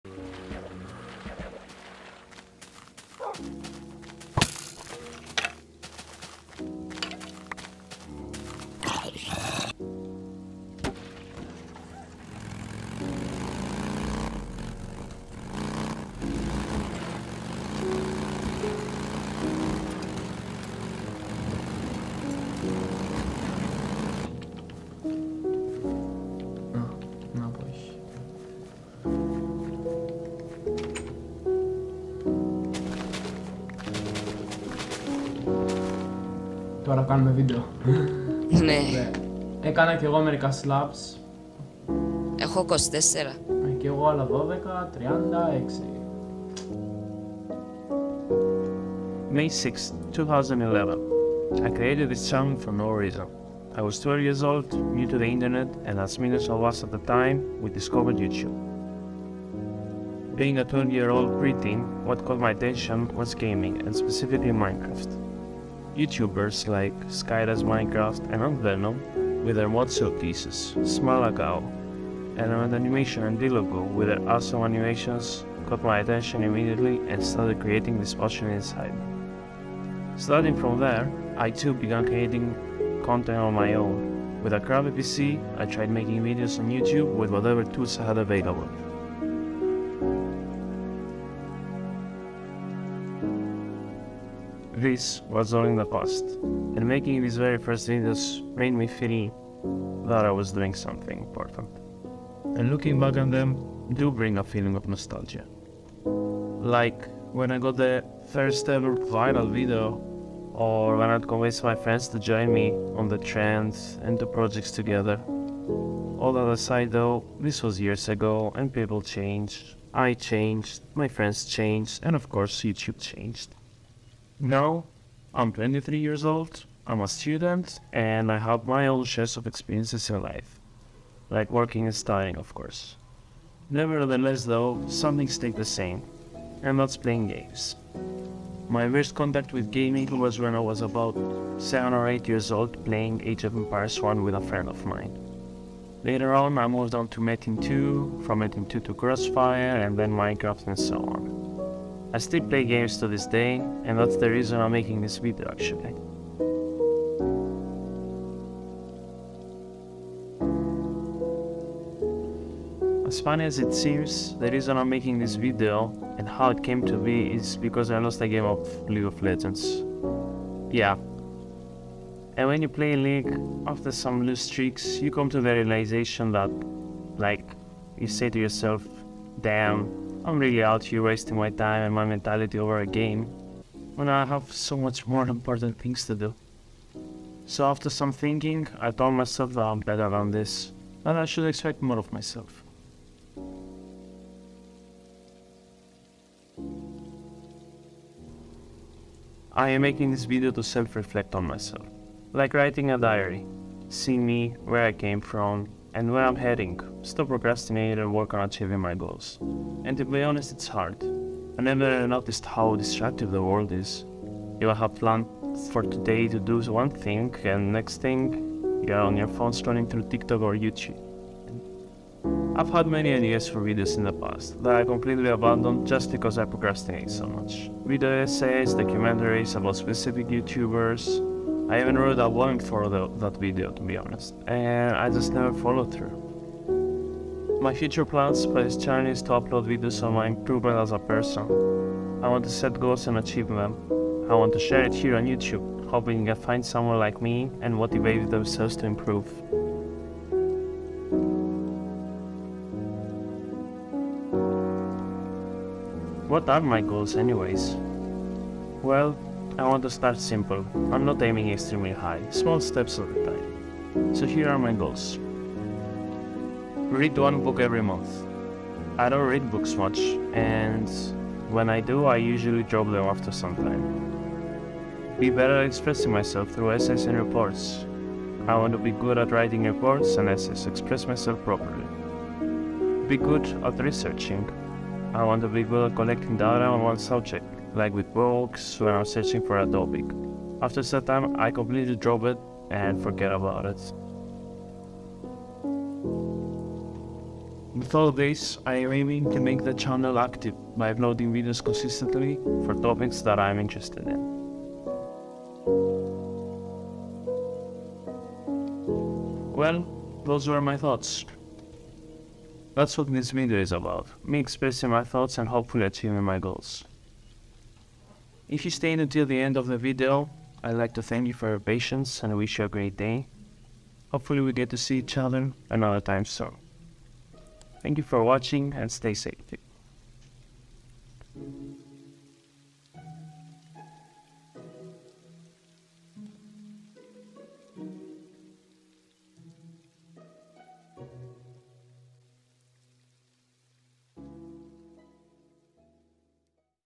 GAMING Through The Road To Self-Improvement, How NOSTALGIA Drives you Closer To Your GOALS
I created this channel for no reason. I was 12 years old, new to the internet and as millions of us at that time we discovered YouTube. Being a 12 year old pre-teen, what caught my attention was gaming, and specifically Minecraft...

Chapters:
0:00 - Intro
1:33 - First Steps, Who Am I?
2:39 - What Is This Video? Future Plans?
4:14 - My Goals
5:13 - Until Next Time